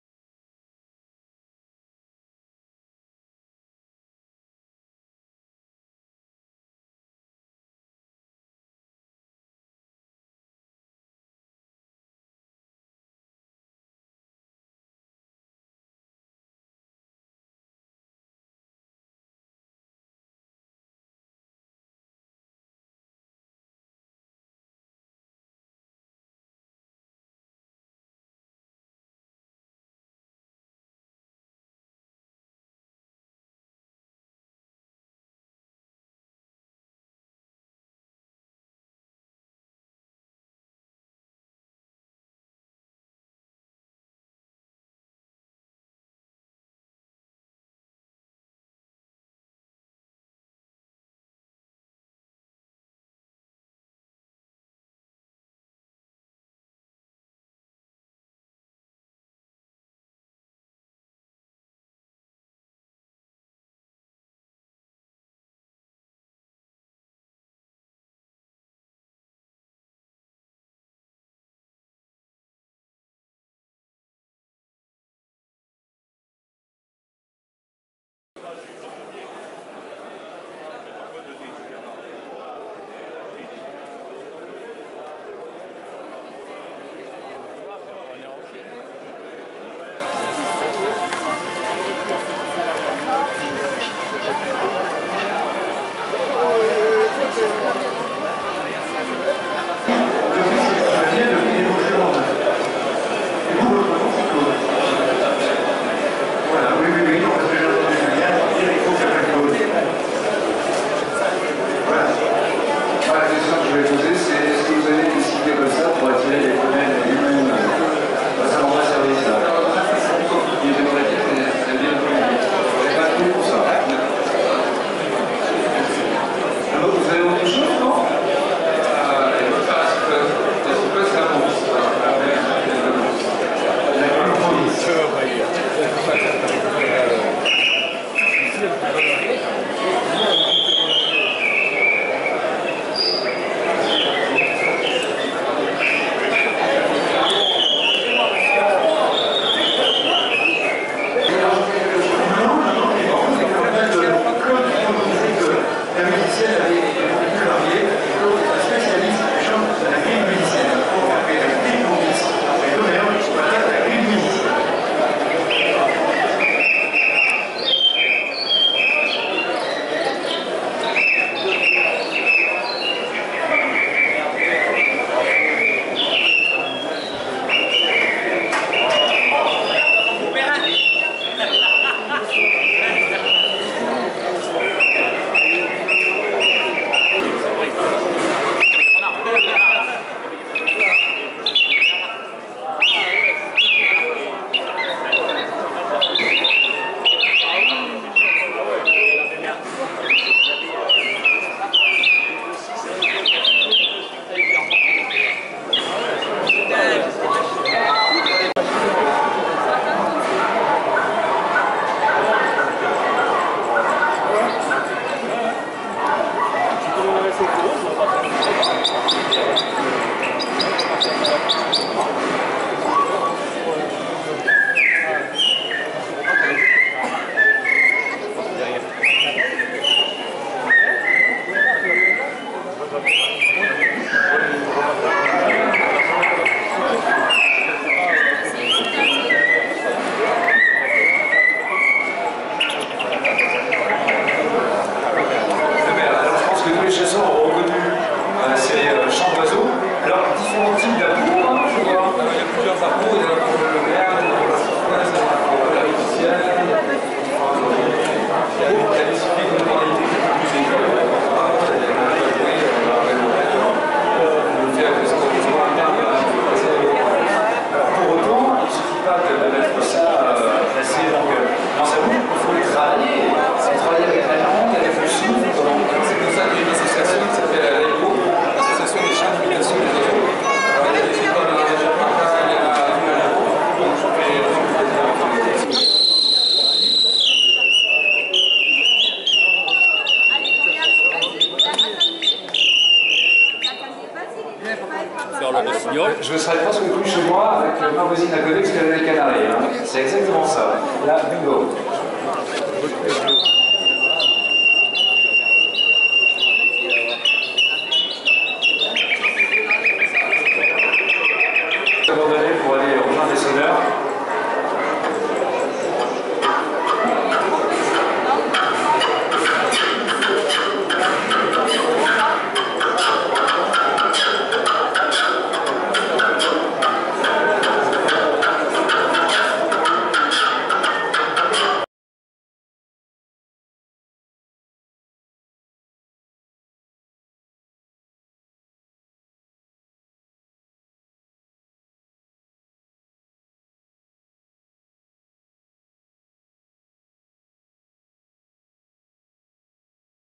Imitation des chants d'oiseaux
Démonstrations des chants d'oiseaux, foire de Saint Gély du Fesc, Hérault, 7 juin 2015